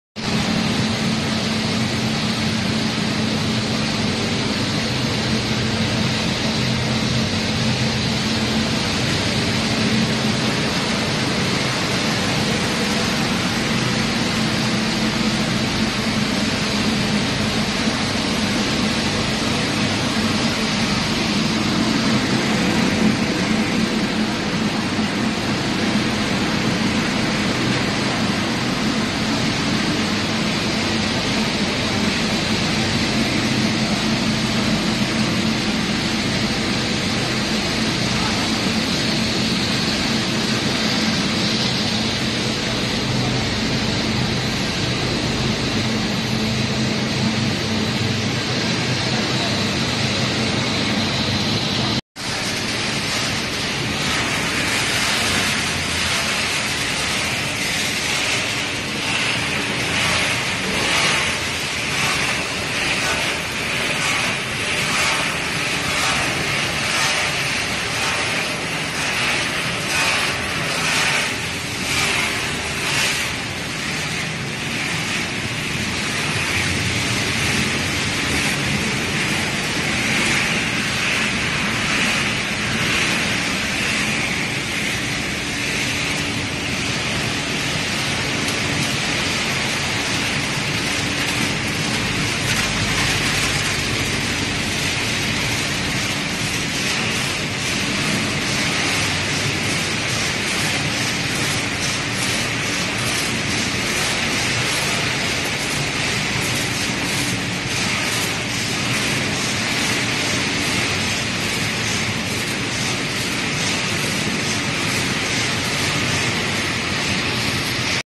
Darin Dogfood Extruder Dog Kibble Extrusion Line Petfood Machine with Packing Machine run in Russia
How It's Made Darin Dogfood Extruder Dog Kibble Extrusion Line Petfood Machine with Packing Machine run in Russia